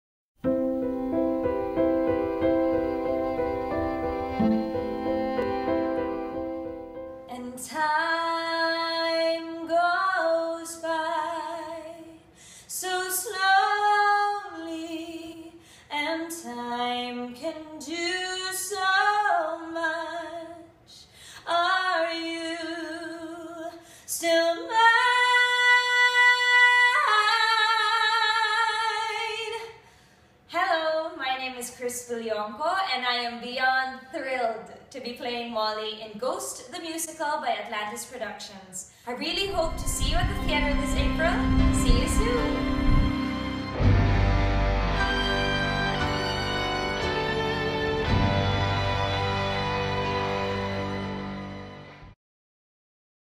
Cris Villonco for GHOST The Musical
GHOST The Musical will be showing from April 25 - May 11 at the Carlos P. Romulo Auditorium, RCBC Plaza, Makati.

For more details call Atlantis Productions at 892 7078.